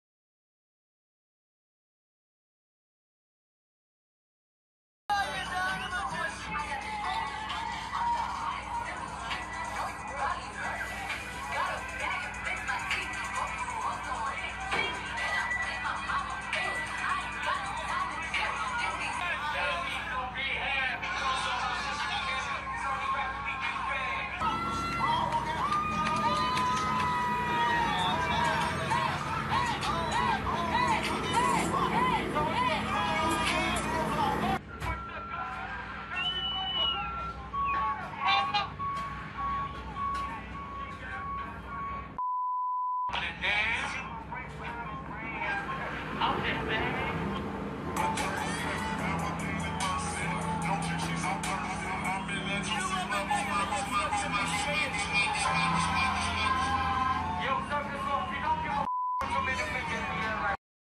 CONOR MCGREGOR and Pals in Raunchy Night out at London Club
Conor McGregor throws wads of cash in air as pal uses girl's bum as pretend PUNCHBAG while partying in club
NUDITY WARNING: McGregor is seen with pals throwing £20 notes as he danced alongside the women as he continues to enjoy life away from the Octagon